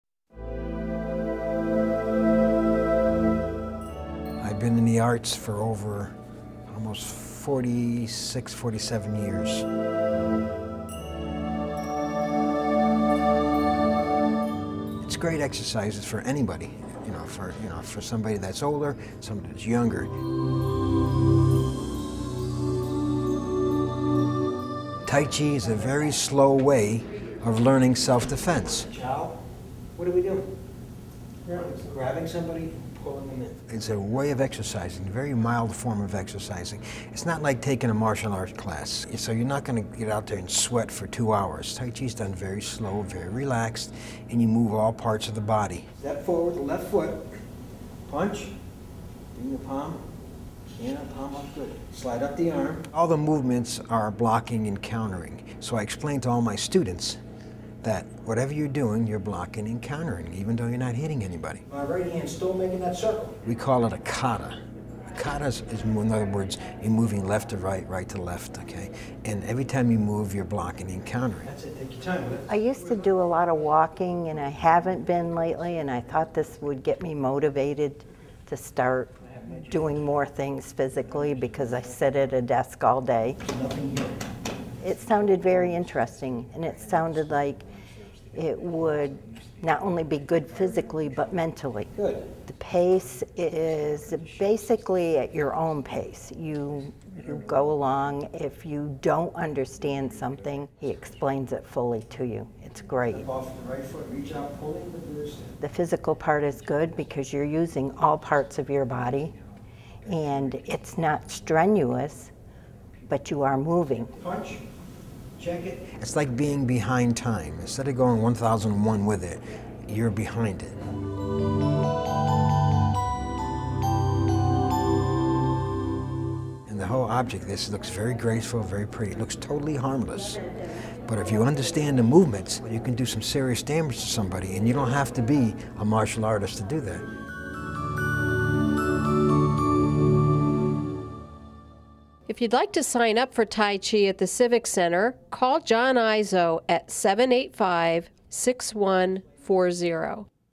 Tai Chi
Good for the mind and body, Tai Chi classes at the Port St. Lucie Civic Center enable students to learn blocking and countering moves during this very mild form of exercise.